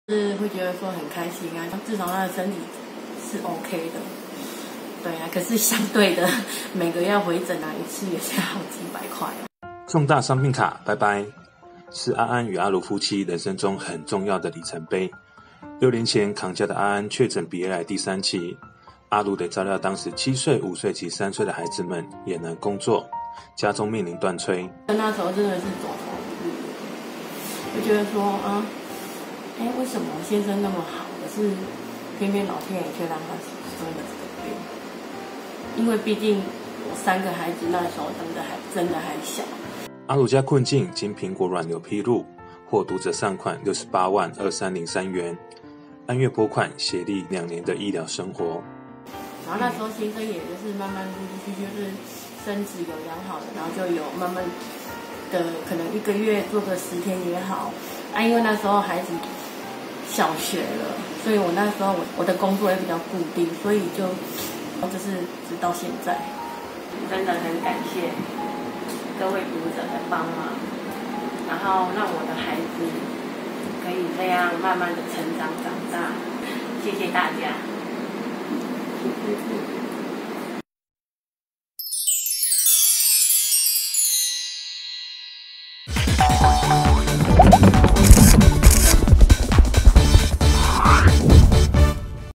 【暖蘋果】揮別「重大傷病卡」 夫妻感恩6年前愛心「改變我們一家未來」 | 蘋果新聞網
「沒了重大傷病卡，先生看病得要多花好幾百元，但說真的，少了這個身分，就是先生成功抗癌的最好證明，夫妻倆都很開心。」36歲阿如(王乙如)邊說話，邊望著先生阿安(石哲安)，兩人互看一會，雙雙笑了起來。